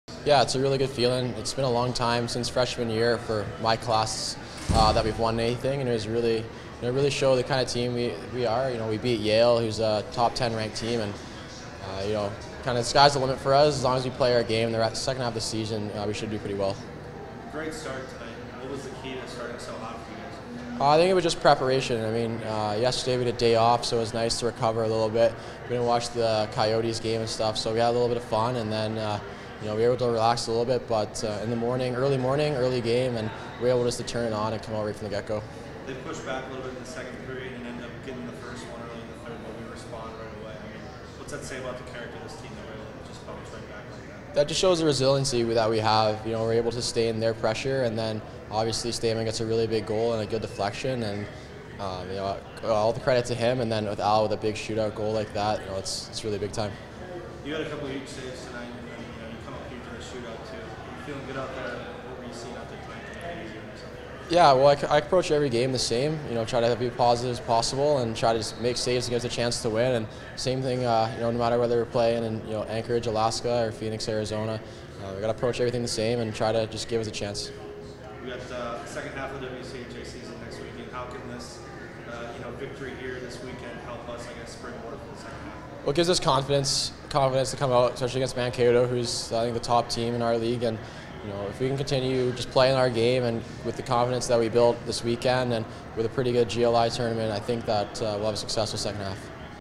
Jamie Phillips Post Game Comments - 1/10/16
Jamie Phillips talks to the media after stopping 24 shots to help the Huskies beat Yale in the Desert Hockey Classic championship.